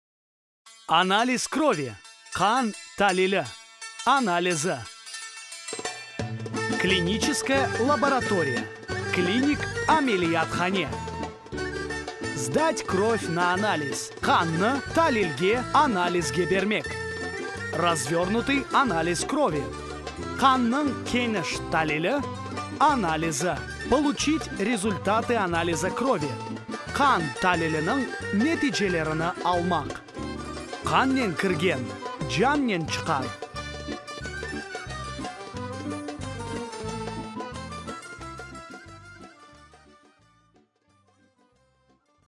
5 главных слов. Анализ крови – Къан талили, анализи (qan talili, analizi)главных слов. Анализ крови
Сегодня в нашей рубрике «5 главных слов» тема необычная, но жизненная. Речь пойдёт об анализе крови – эту процедуру врачи рекомендуют проходить регулярно. Потому что обследования – лучший способ избежать серьёзного и дорогостоящего лечения.